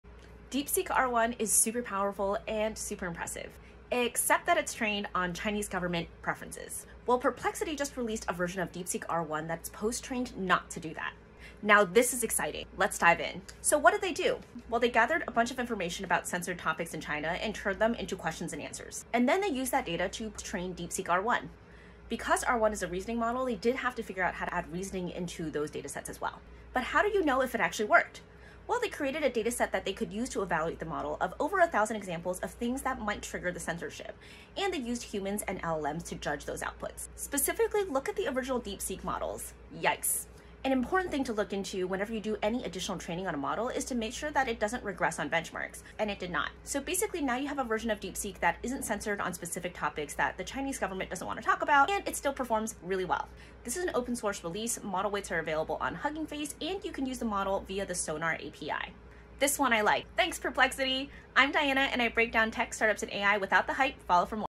Deepseek R-1 1776 trained to be less "censored"
Deepseek R-1, though powerful and impressive, was initially aligned with Chinese government preferences.

Exciting news! Perplexity's latest version is post-trained to break free from that bias!

They compiled data on censored topics in China for post-training.

Results are striking — censorship has dramatically dropped! 👍

Model weights available on Hugging Face or via Sonar API.

Kudos to Perplexity for this one!